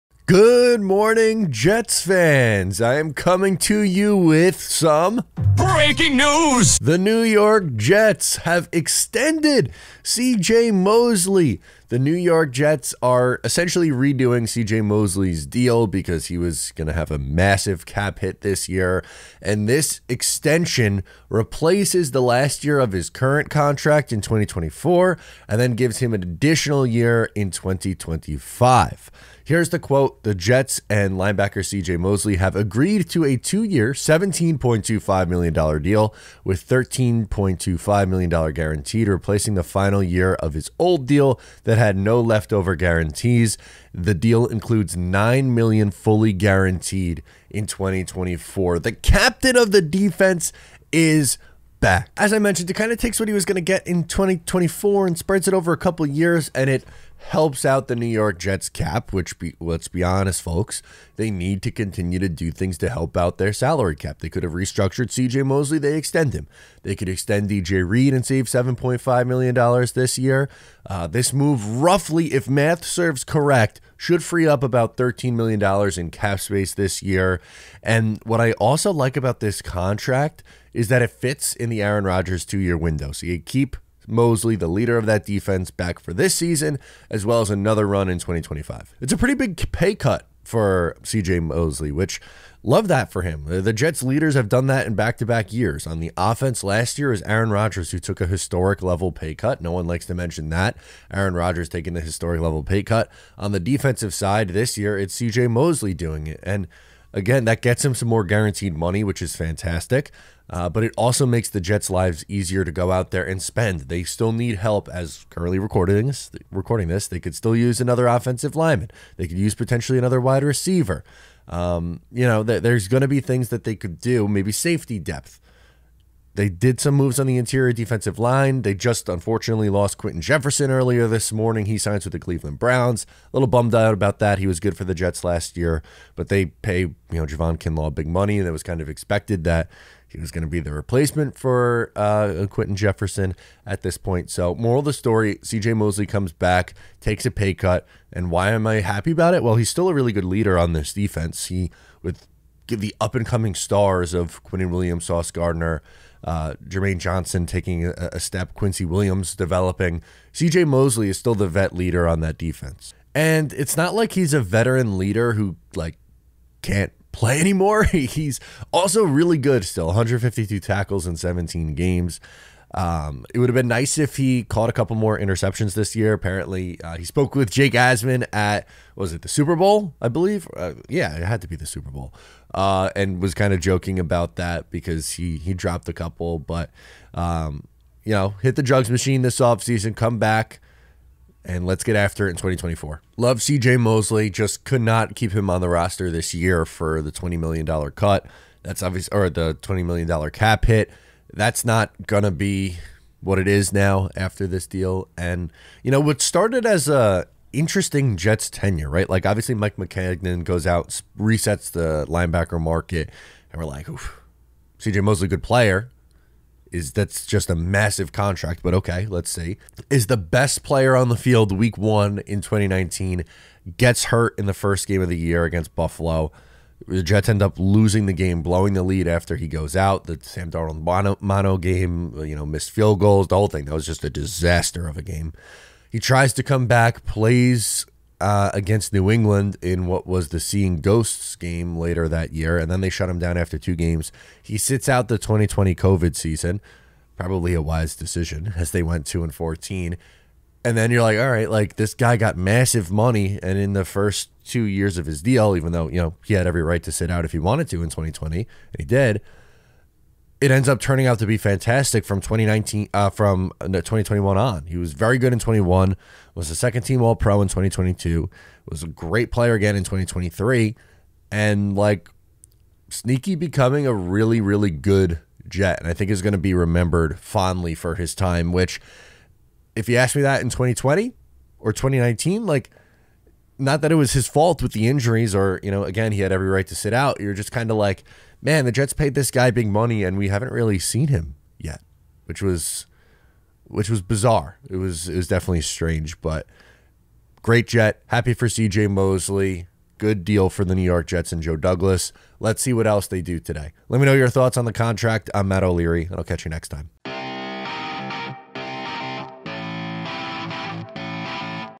NEW YORK JETS EXTEND CJ MOSLEY
Matt O'Leary reacts to the new CJ Mosley contract and how it impacts the New York Jets cap space in a positive way for 2024

🎫 Come hang with us for our first ever Talkin Jets NFL Draft party at the Main Event in Farmingdale, NY

ALWAYS LOYAL SHIRT: